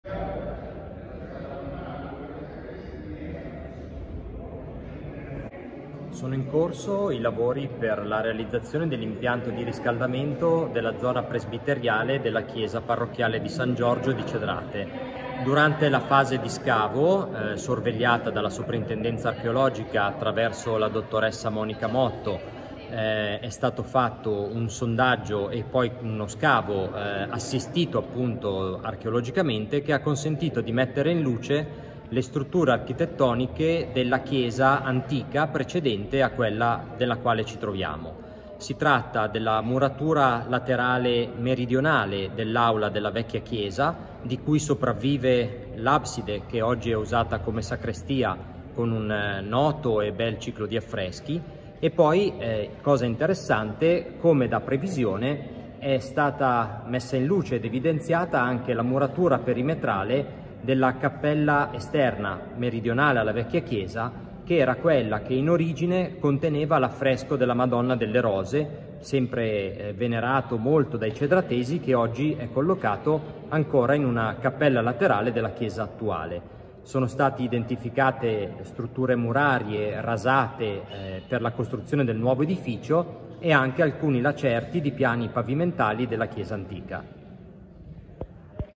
Dentro la chiesa di Cedrate di Gallarate ricompare l'edificio quattrocentesco
Le immagini inedite dello scavo archeologico dentro la chiesa di San Giorgio a Cedrate, che ha restituito un pezzo dell'antico paese: le tracce dei muri della chiesetta ma persino un tratto di "marciapiede" esterno